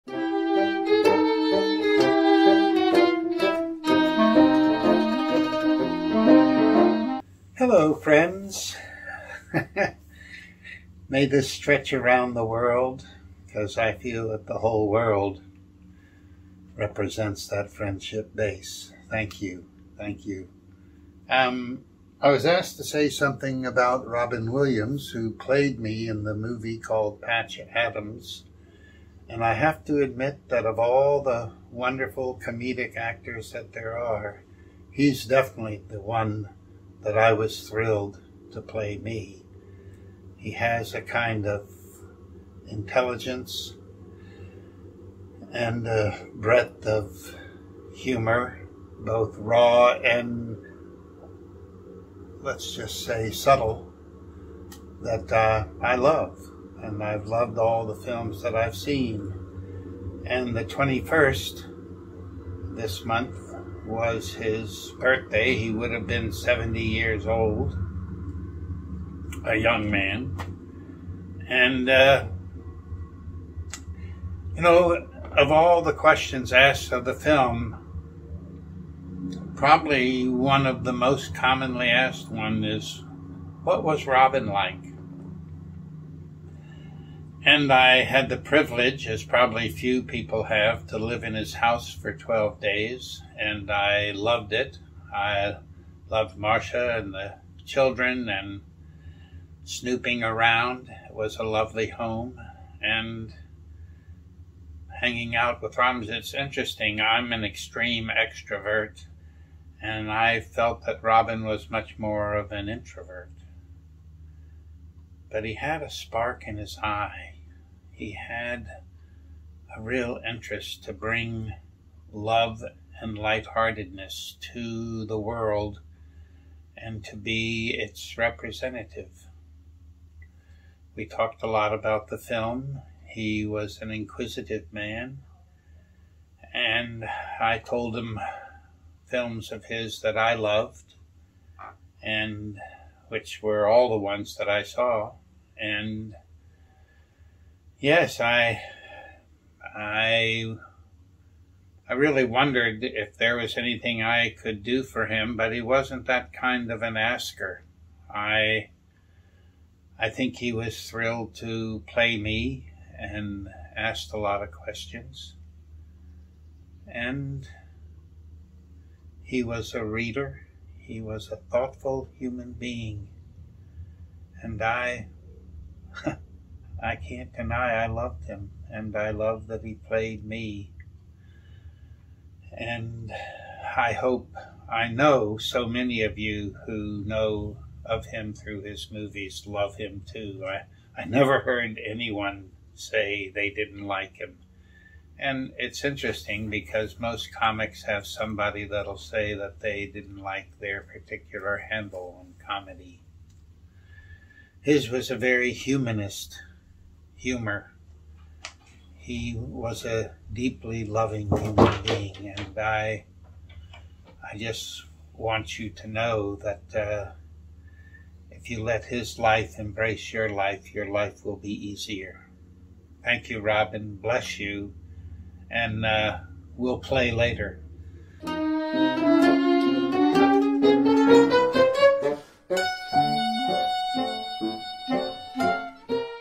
To My Friend, Robin
I wanted to do a video today to honor Robin Williams, whose 70th birthday would have been last week. We all miss him. However, people we miss are never truly gone. As long as you remember them, their memory will live on. What is your fondest memory of Robin? What is your favorite Robin Williams movie?

In peace,
Patch Adams, MD